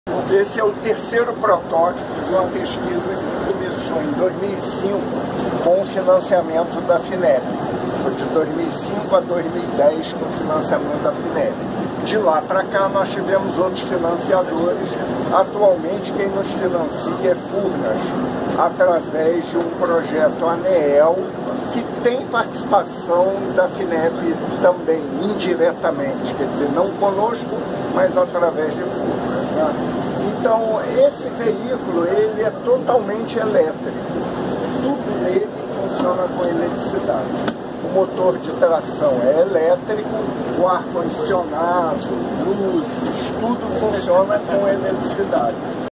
Ônibus movido a hidrogenio
O ônibus movido a hidrogênio, projeto da Coppe UFRJ, tem apoio da Finep. No vídeo, o secretário-executivo do Ministério da Ciência, Tecnologia, Inovações e Comunicações, Elton Zacarias, e o presidente da Finep, Marcos Cintra conhecem o veículo ao vivo.